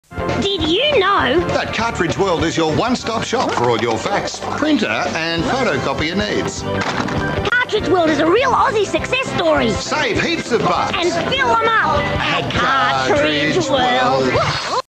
Cartridge World Mt Gambier - 15sec Television Commercial, 2006
During the Redbull Gives you Wings Air-series in Perth on WIN Ten Mt Gambier.